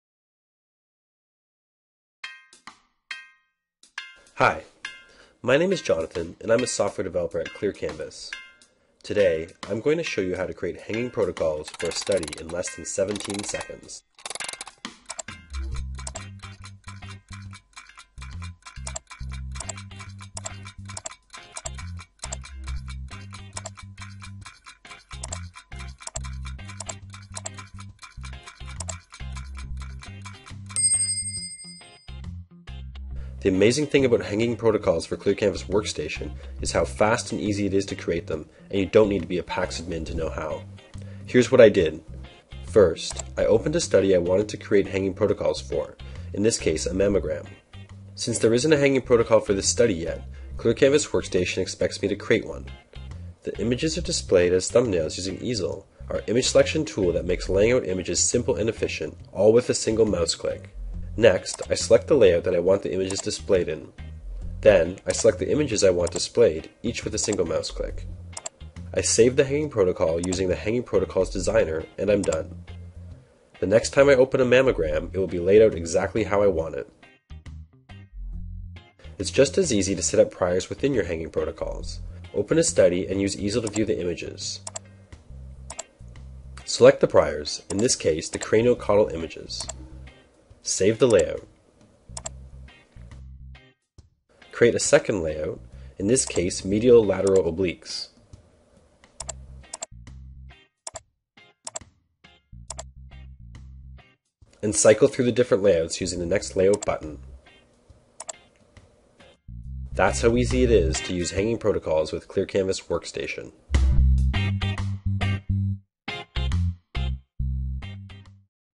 ClearCanvas Workstation: Hanging Protocols
ClearCanvas Workstation is a powerful and easy to use PACS Workstation. This video demonstrates the Hanging Protocols feature of ClearCanvas Workstation. Hanging Protocols is currently only available as part of ClearCanvas Workstation, Team Edition.